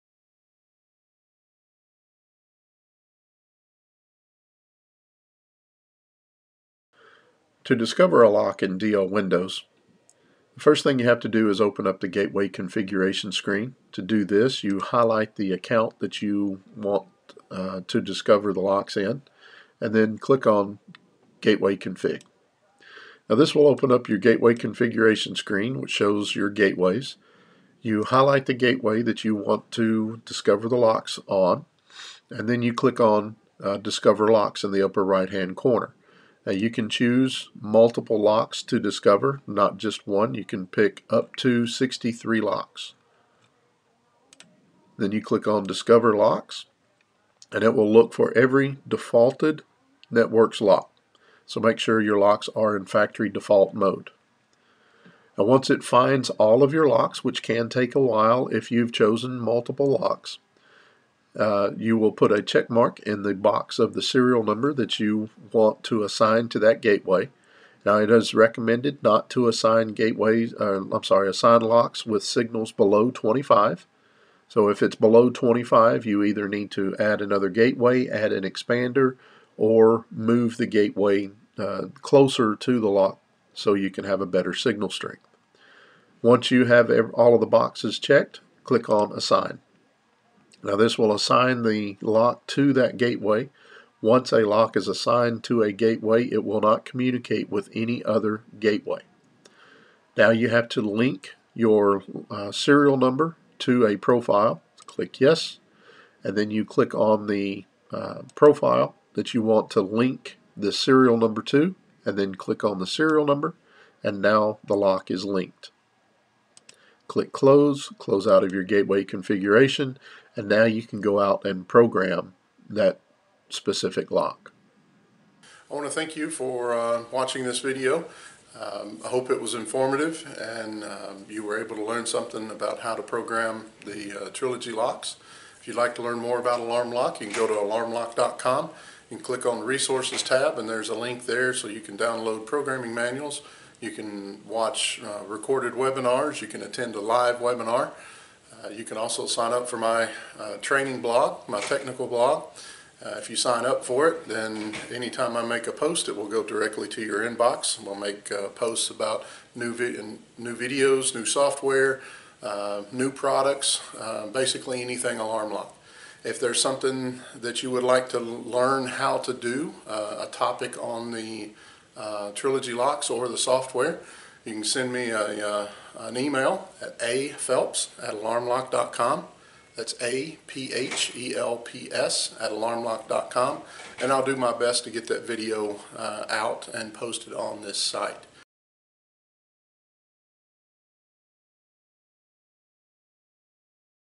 Discovering a Networx lock in DL Windows 5.5.3
Andy Phelps, Alarm Lock's National Training manager, shows you how to discover a Networx lock in the DL Windows Trilogy lock programming software.